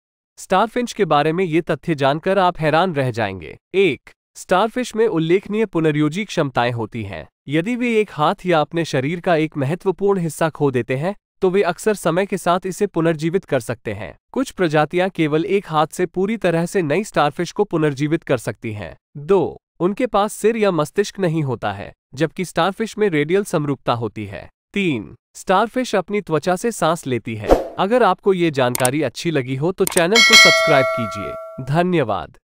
Unknown facts about Starfish
Starfish, also known as sea stars, are fascinating creatures with several lesser-known facts and unique characteristics:

1. Starfish have remarkable regenerative abilities. If they lose an arm or even a significant portion of their body, they can often regenerate it over time. Some species can even regenerate an entirely new starfish from just a single arm.

2. While starfish have a radial symmetry, they don't have a head or a brain.

3. They push their stomach into the shell of their prey, releasing digestive enzymes to break down the mollusk's tissues, and then absorb the liquefied nutrients.

4. Starfish breathe through their skin.